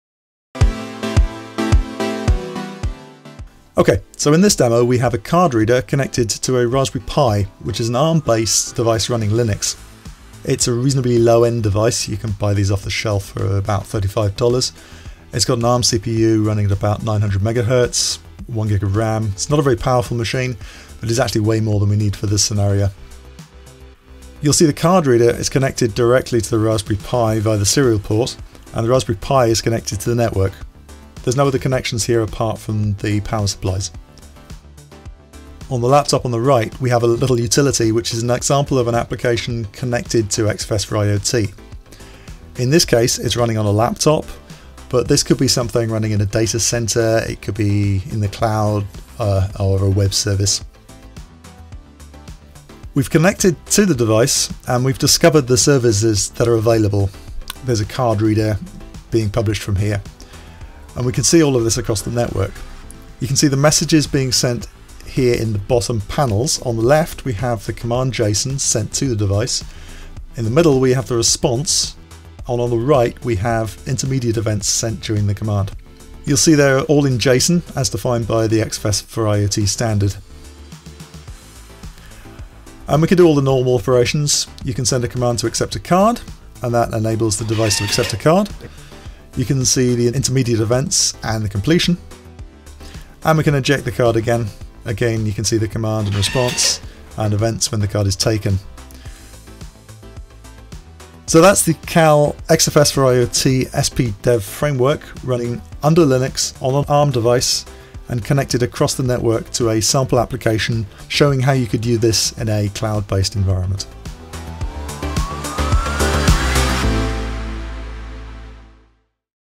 KAL XFS4IoT SP-Dev Framework on Linux
XFS4IoT is the new major version of the XFS global standard for ATMs and a major leap forward when compared with XFS version 3.

In this video, we demonstrate that the new standard is OS and hardware agnostic by testing it on a low-cost, credit-card sized computer, the Raspberry Pi.